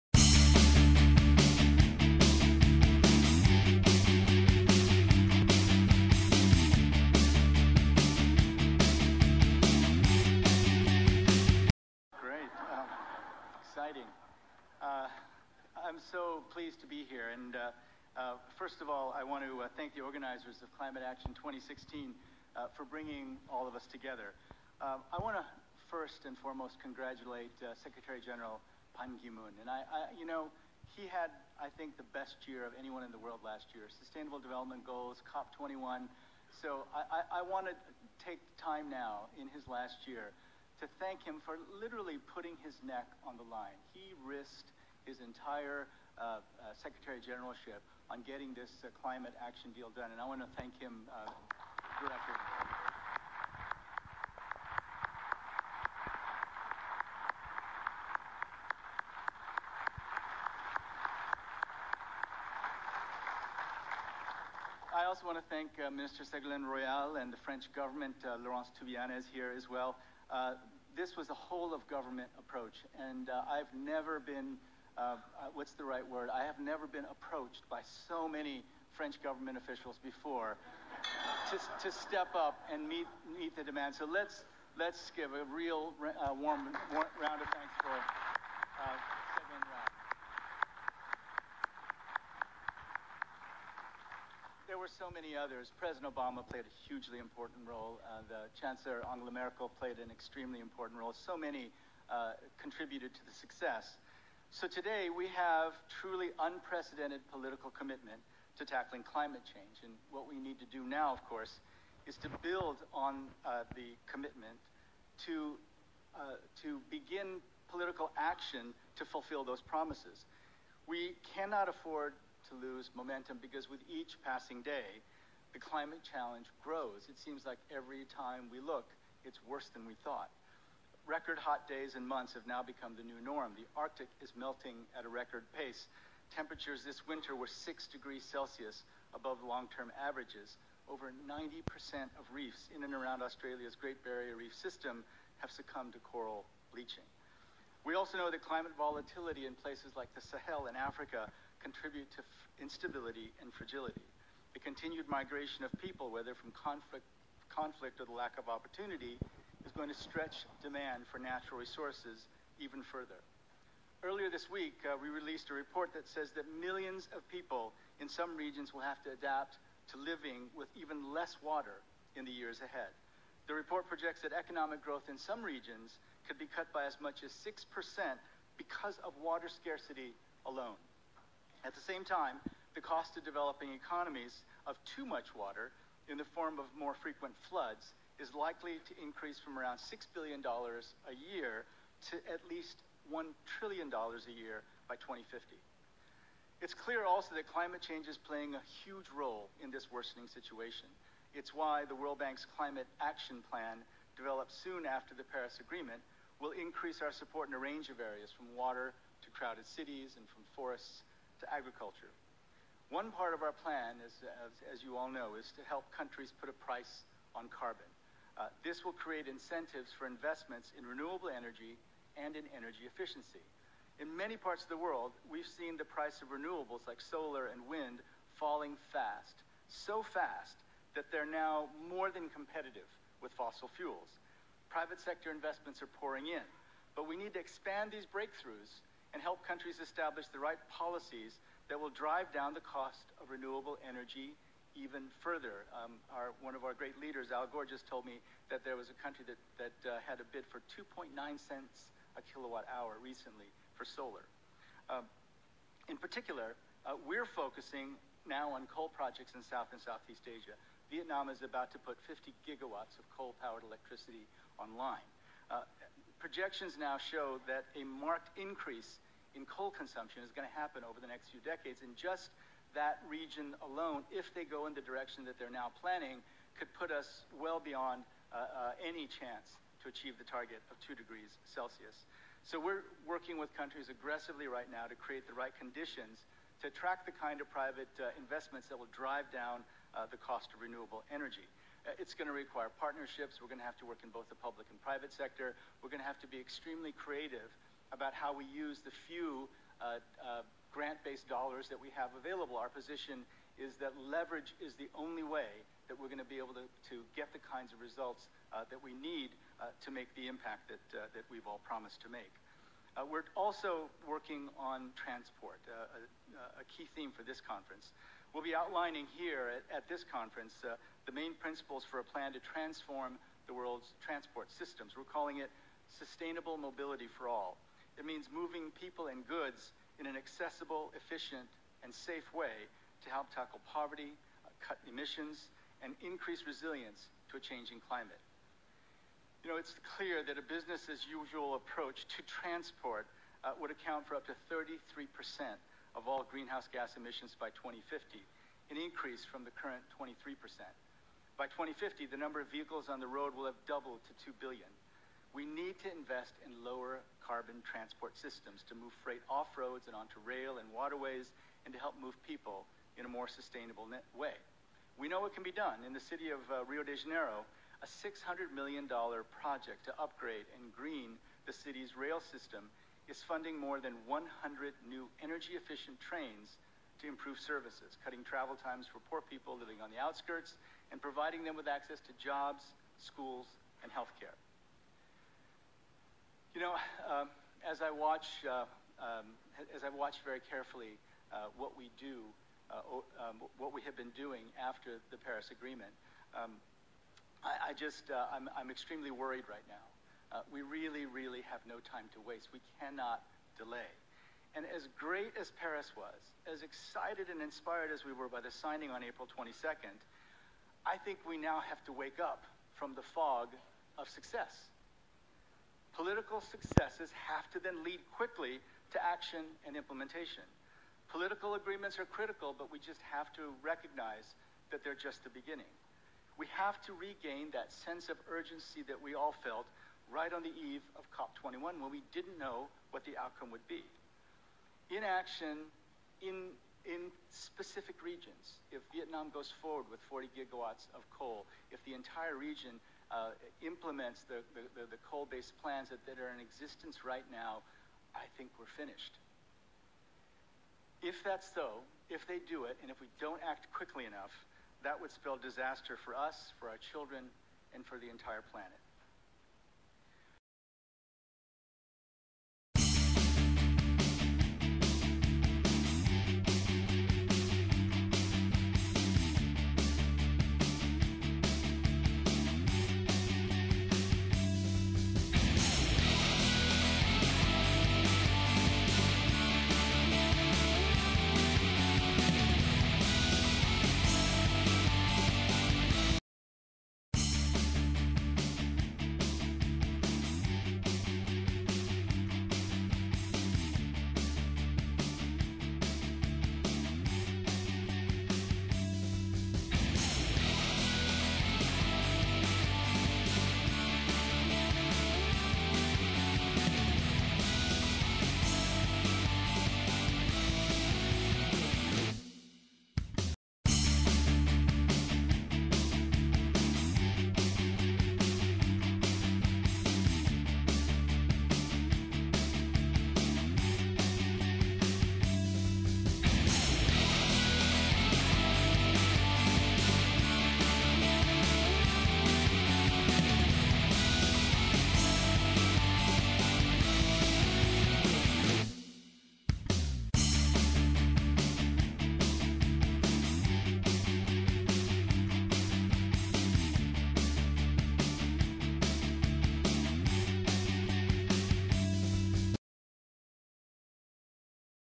Climate Action 2016 World Bank President @World Parliament (IPU) Washington D.C. 5/5/2016
NWO The New World Order in all it's glory.
Inter-Parliamentary Union (IPU)
Conference of Speakers of Parliament 5/5/2016

Jim Yong Kim, World Bank President.
Climate Action 2016 May 5th Washington D.C.

Climate Action 2016.
World Conference of Speakers of Parliament.